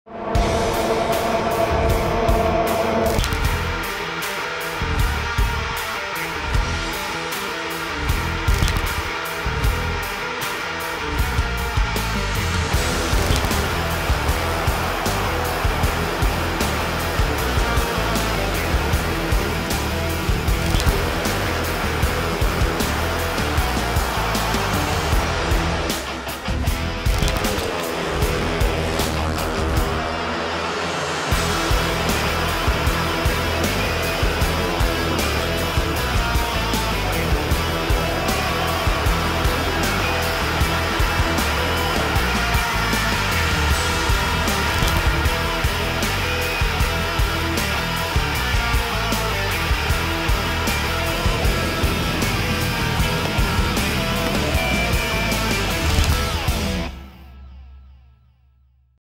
ARCHIVE: The 2006 British GP | FIM Speedway Grand Prix
Back in 2006, FIMSpeedwayGP legend Jason Crump claimed victory in the BritishGP and Cardiff at the Principality Stadium.